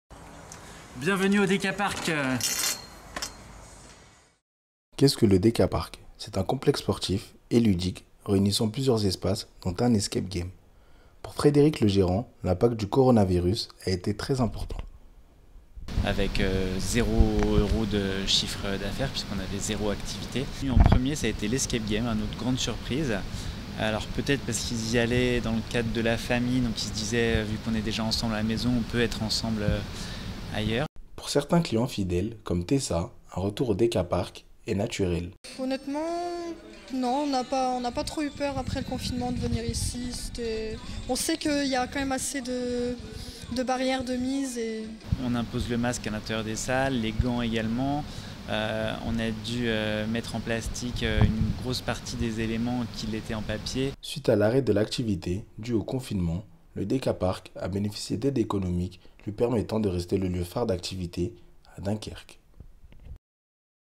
Reportage - Escape game
Au coeur du lieu phare d'activité de Dunkerque.

Reportage d'Amadou Cissé, Christian Matiafu et Elsa Yegavian